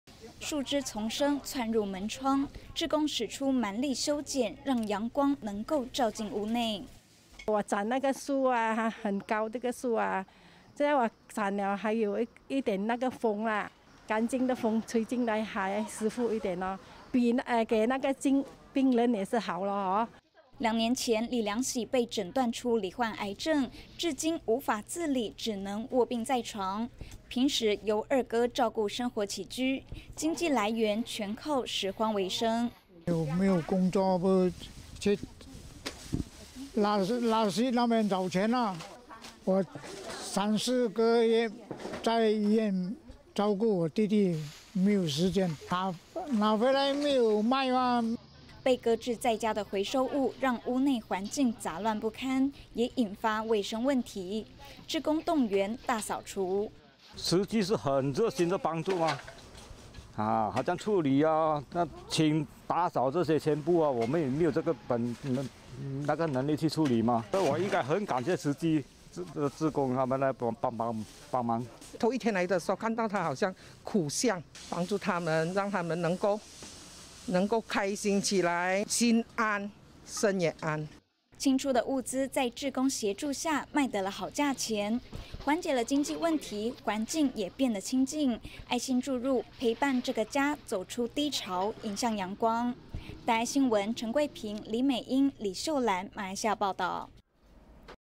20201030【慈善】幸福路上陽光普照 守護大馬兩兄弟的家
兩兄弟相依為命，兩年前李良喜被斷患有癌症，生活無法自理，全由二哥照應。二哥平時靠撿拾回收物變賣維生，如今要照顧弟弟，分身乏術，留著滿屋的回收物，無暇繼續賣錢，只好求助慈濟志工，協助清掃。
樹枝叢生，竄入門窗，志工使出蠻力修剪，讓陽光能夠照進屋內。

慈濟志工 姚美蓉：「我斬那個樹 很高這個樹，這樣我斬了 還有一點風，乾淨的風吹進來還舒服一點，給那個病人也是好。」

兩年前，李良喜被診斷出罹患癌症，至今無法自理，只能臥病在床，平時由二哥照顧生活起居，經濟來源，全靠拾荒維生。

李良喜二哥 李亞利：「沒有工作 去垃圾那裡找錢，我三 四個月在醫院照顧我弟弟，沒有時間 拿回來沒有賣。」

被擱置在家的回收物，讓屋內環境雜亂不堪，也引發衛生問題，志工動員，大掃除。

李良喜三哥 李良發：「慈濟是很熱心地幫助，處理打掃這些全部，我們也沒有這個能力去處理，所以我很感謝慈濟志工來幫忙。」

慈濟志工 戴桂梅：「頭一天來看他好像苦相，幫助他們 讓他們能夠開心起來，心安 身也安。」

清出的物資，在志工協助下，賣得了好價錢。緩解了經濟問題，環境也變得清靜，愛心注入，陪伴這個家，走出低潮，迎向陽光。

馬來西亞報導：陳桂屏 李美英 李秀蘭